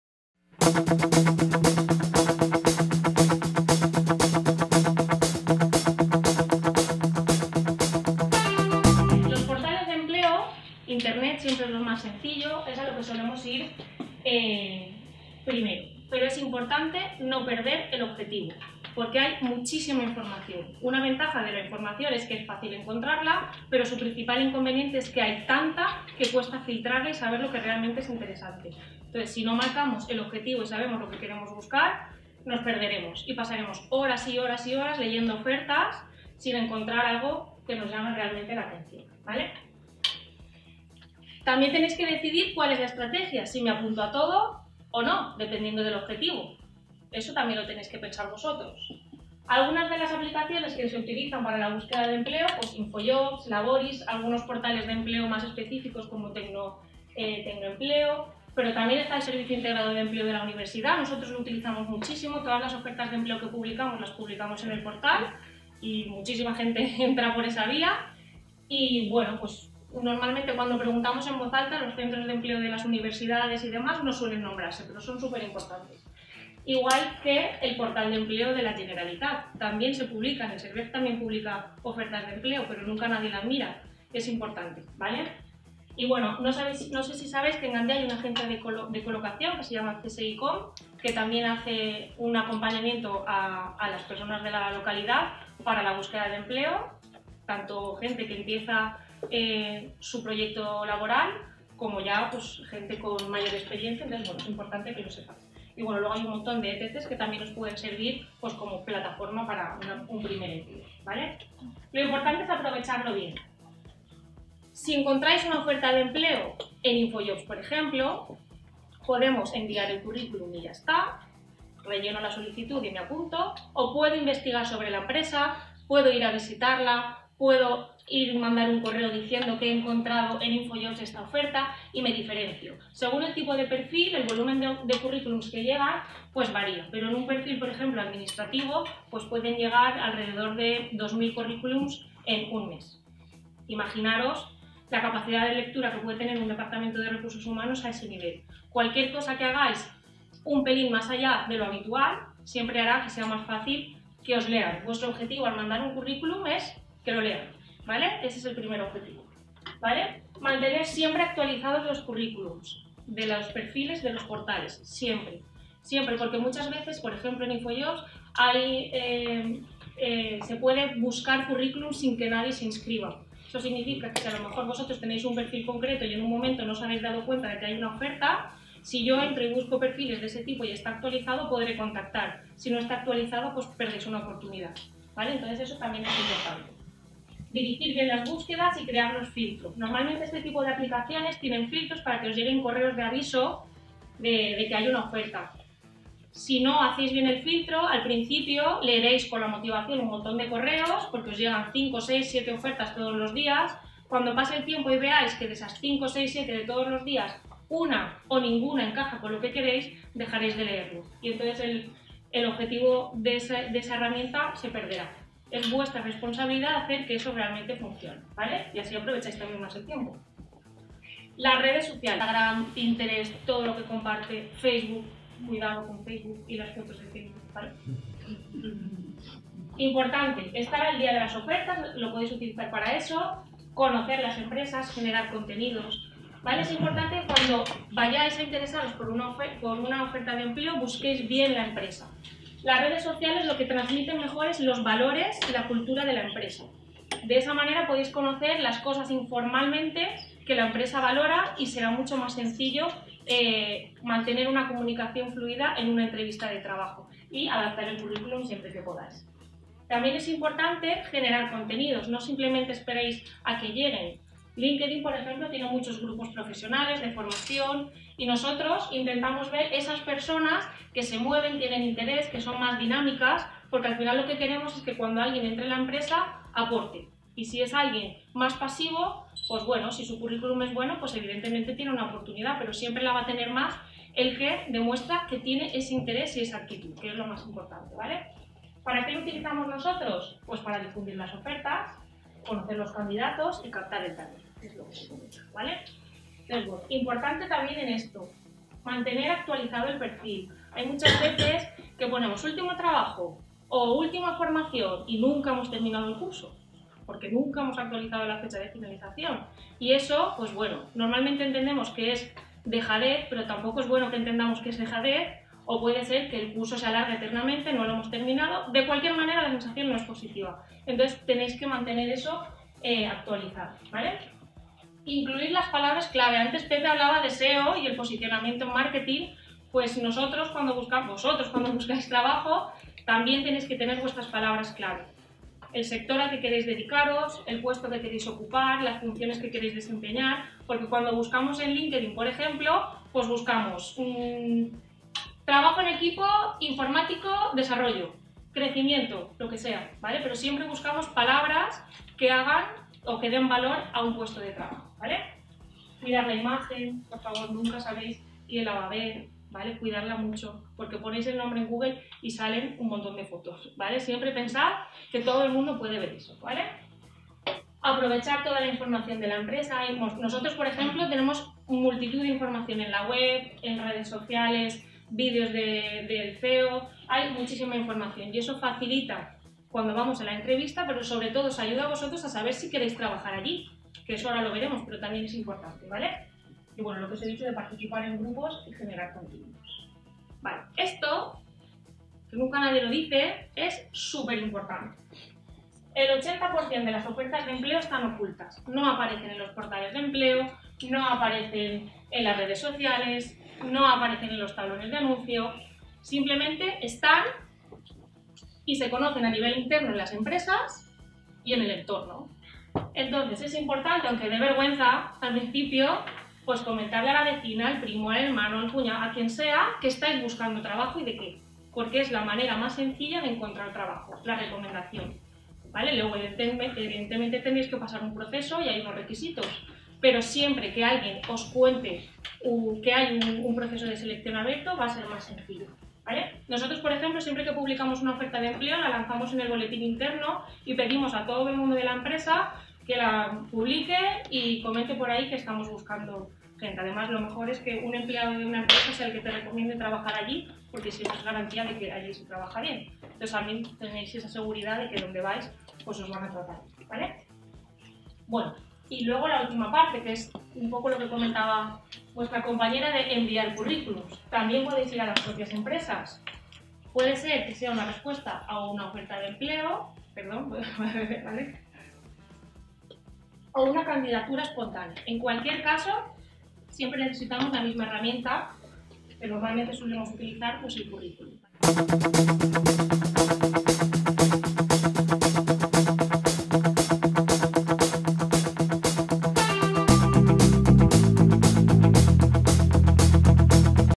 Planifícate para buscar empleo
A la hora de buscar trabajo es muy importante la planificación. Saber qué hacer en cada momento es crucial para que los seleccionadores escojan tu currículum. En el siguiente vídeo, la Directora de Recursos Humanos de La Tienda HOME explica cuales son las pautas de planificación para buscar empleo y qué debemos tener en cuenta a la hora de hacerlo.